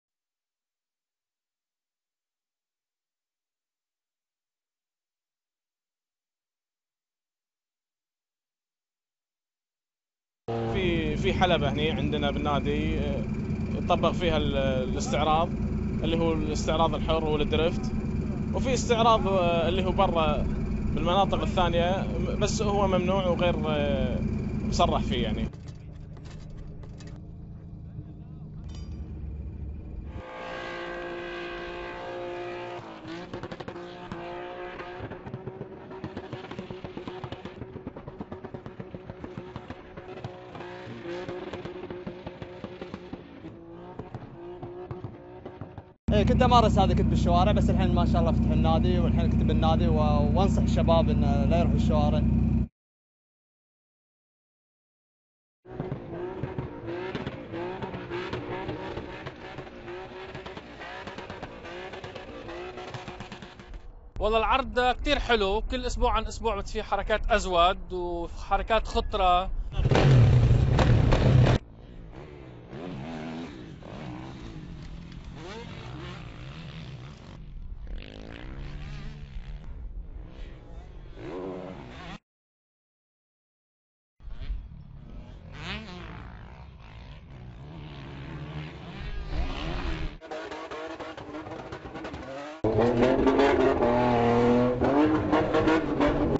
Kuwaiti Extreme Driving
Illegal street racing, is becoming safer as drivers move from the streets to special tracks